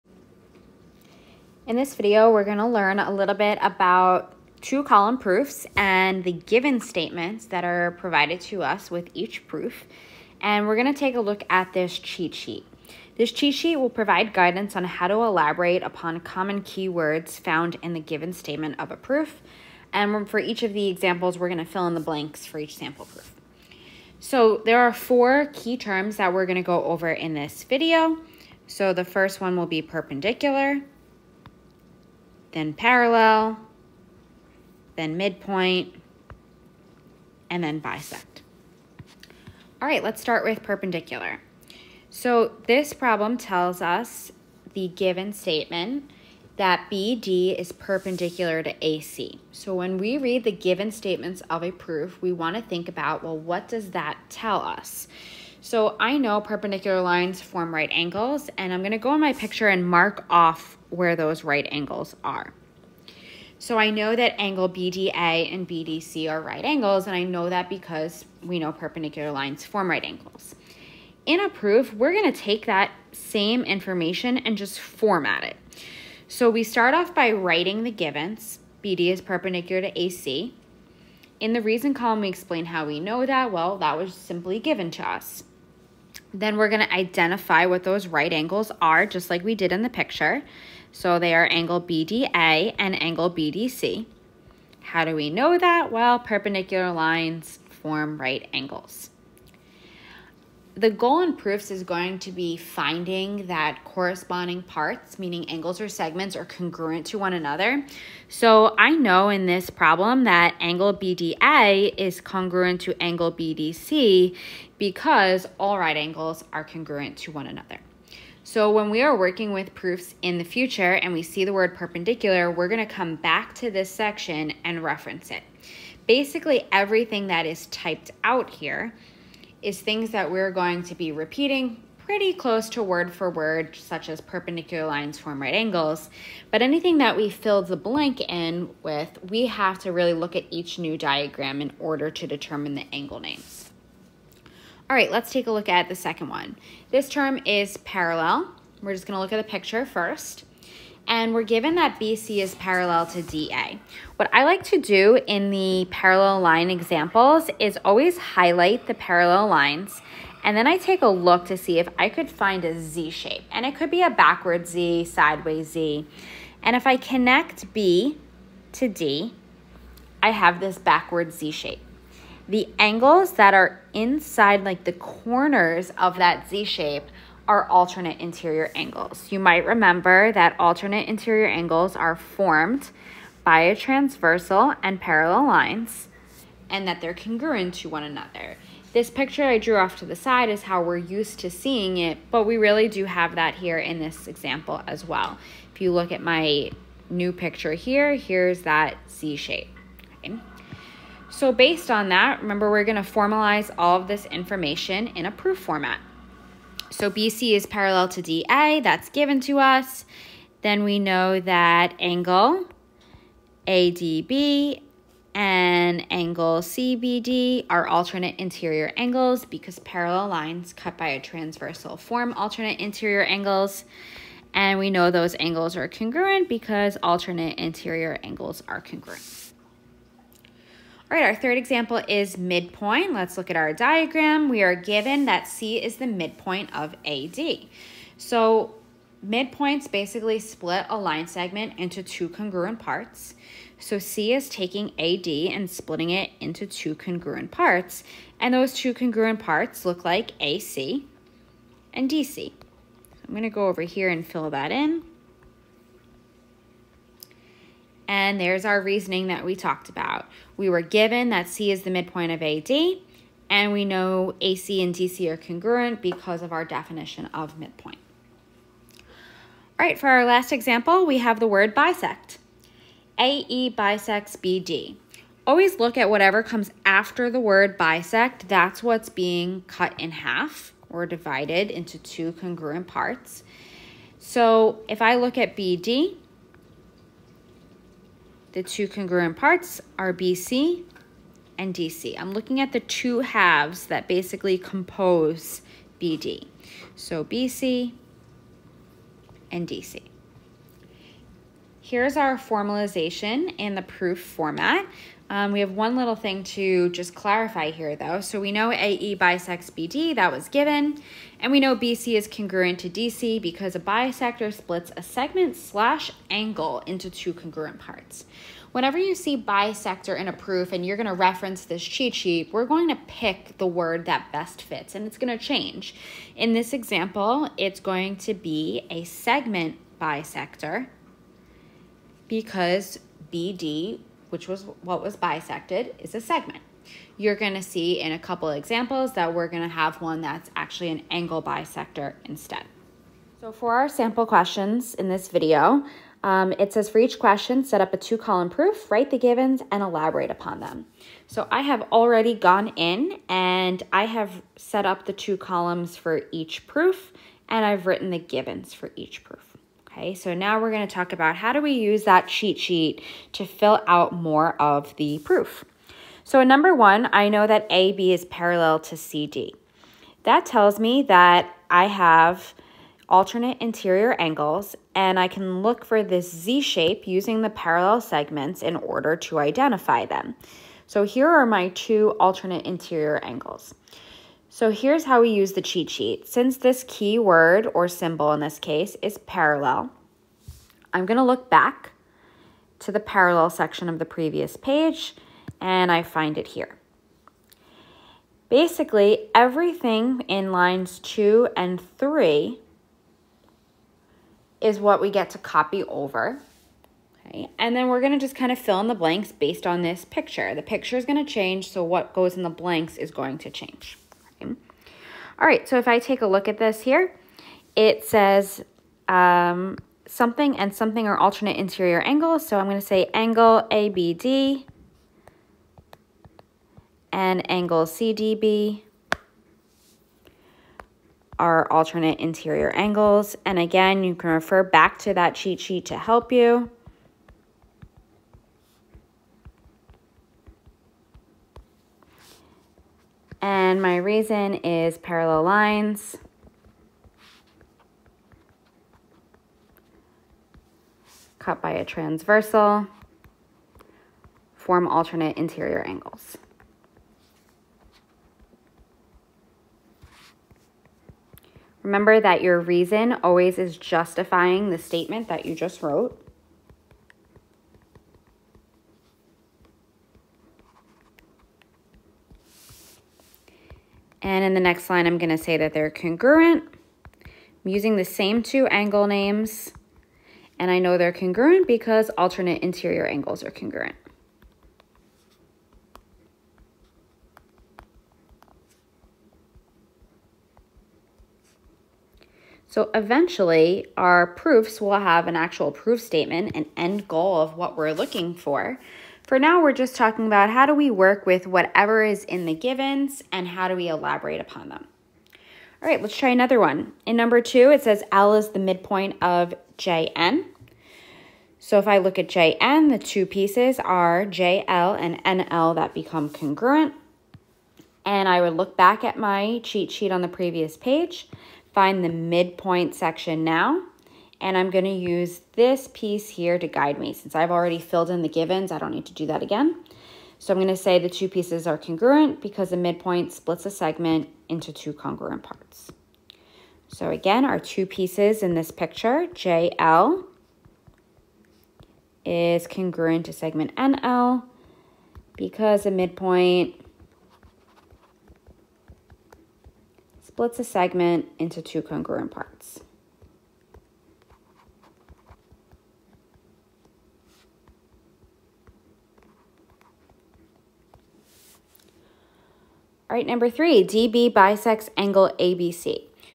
The Givens "Cheat Sheet" | High School Geometry Lesson
This video will teach students how to elaborate upon key words in the givens for congruent triangle proofs: parallel, perpendicular, midpoint, and bisect.

Grab the notes that go with the video (along with additional practice) by clicking